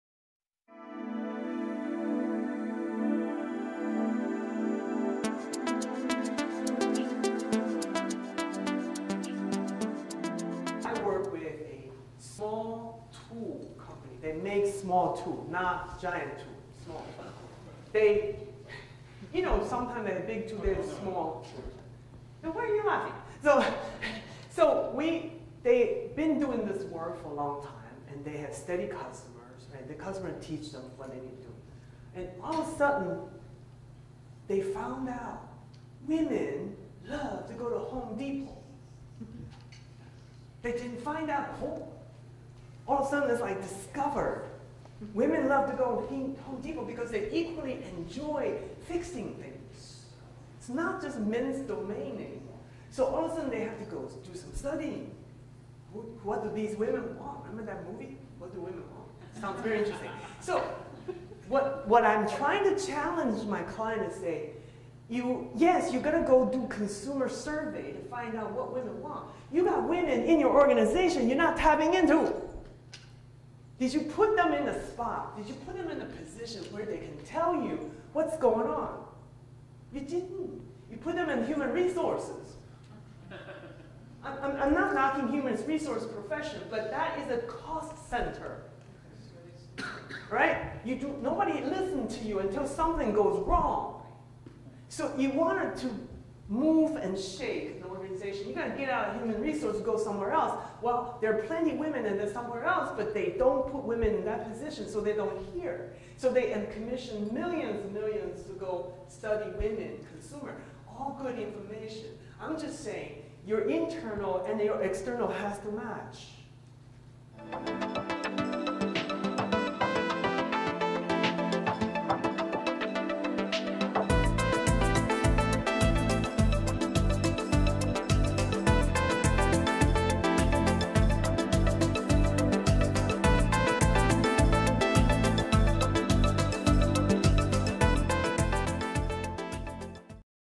Letting Your Diverse Employees Lead You To Diverse Customers:
Organizations spend millions of dollars to study their emerging and diverse markets, but rarely tap into the internal resources they have right under their noses their diverse employees! Celia challenges todays organization to balance the internal with the external, by not only hiring diverse talents but also putting them in positions where their ideas can be heard.